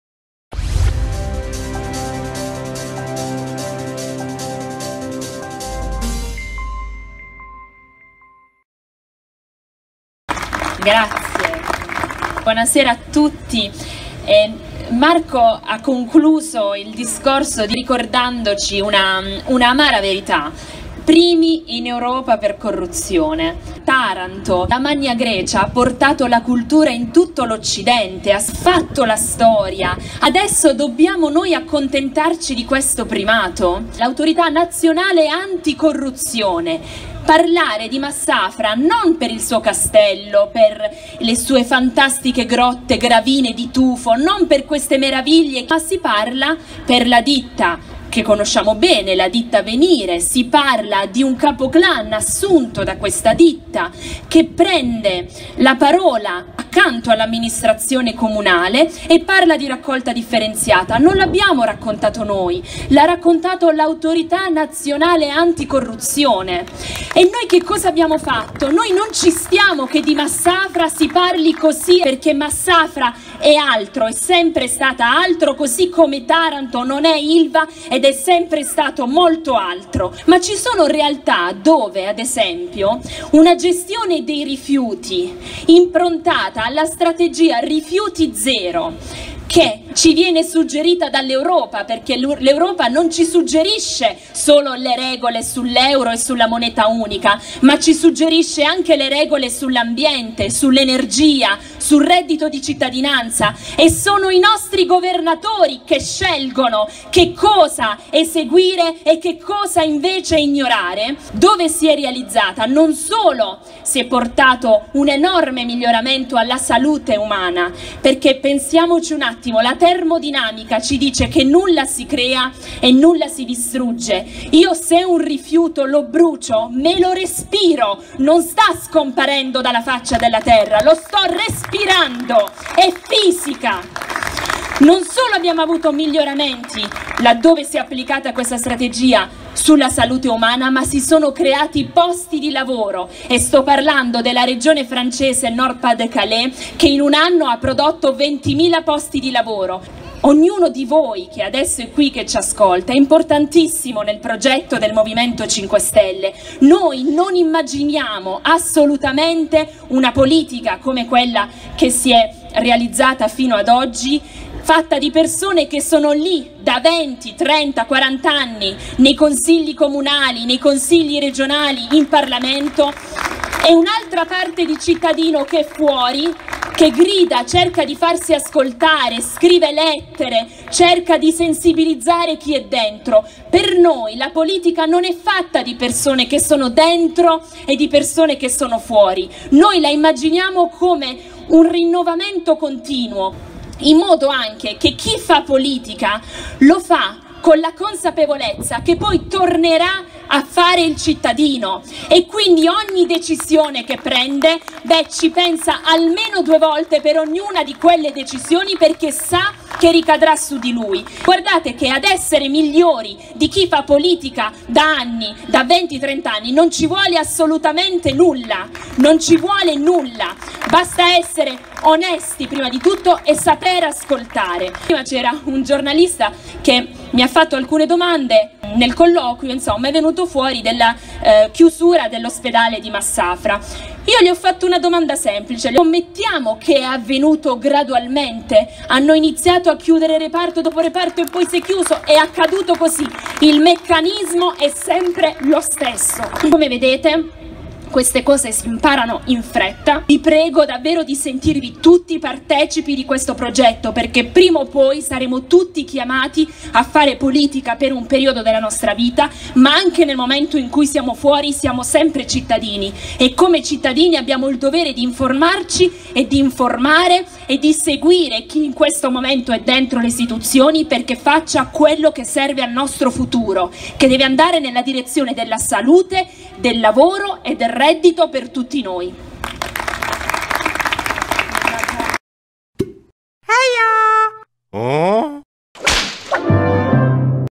Antonella Laricchia - 21 novembre 2015 - Massafra (TA) - No Mafia Day
UN SABATO DA M5S, TRA IL NoMafiaDay A MASSAFRA, UNA PUBBLICITÀ FRATRICIDA E LE SOLITE PAROLE VUOTE DEI VECCHI PARTITI!

Ieri, sabato 21 novembre 2015, la città di Massafra ha visto la sua cittadinanza scendere in piazza per gridare il proprio sdegno nei confronti della mafia e di un fare mafioso troppo spesso diffuso all'interno della vecchia politica. Massafra, come Taranto, come molte altre realtà della nostra Puglia, è stanca di essere conosciuta per i suoi fatti di cronaca, mala politica o cattiva gestione dei rifiuti e vuole liberarsi da un sistema di sviluppo che crea solo morte e disoccupazione, tornando a riappropriarsi della sua identità storica, turistica, ambientale e imprenditoriale. Uno splendido incontro dove, tra le altre cose, abbiamo anche raccolto nuove istanze da portare in Consiglio Regionale.

Ma la giornata di ieri è stata anche segnata dalla messa in onda dello spot, di un oleificio di Ginosa, che in maniera aberrante invitava a consumare il proprio olio perché "il Salento ora ha la Xylella, le Gravine hanno gli ulivi sani, il nostro olio è sano, noi non siamo il Salento." Oltre a vergognarmi profondamente, da pugliese, di questa guerra fratricida per meri interessi economici, voglio ricordare, come ho già detto qualche giorno fa che grazie ad una richiesta del nostro Portavoce alla Camera Giuseppe L'Abbate - Cittadino a 5 Stelle, l'Accademia Nazionale dell'Olivo e dell'Olio ha sancito che l’INFEZIONE DA XYLELLA FASTIDIOSA NON PREGIUDICA LE QUALITÀ CHIMICHE E ORGANOLETTICHE DEGLI OLI ESTRATTI DAGLI ALBERI INFETTI.

Naturalmente e giustamente la politica, compreso il nostro Presidente di Regione, non ha perso occasione per strapparsi le vesti e battersi il petto per questo avvenimento, schierandosi a difesa degli olivicoltori e produttori di olio del Salento. Ottimo, ora però alle parole, tante, facciano seguire i fatti. La Regione è corresponsabile del taglio indiscriminato degli ulivi salentini. Ponga fine a questa inutile mattanza portando in Consiglio Regionale e votando la mozione del M5S.

Vediamo se alle parole faranno seguito i fatti.

P.S. Nel video una sintesi del mio intervento a Massafra per il NO MAFIA DAY.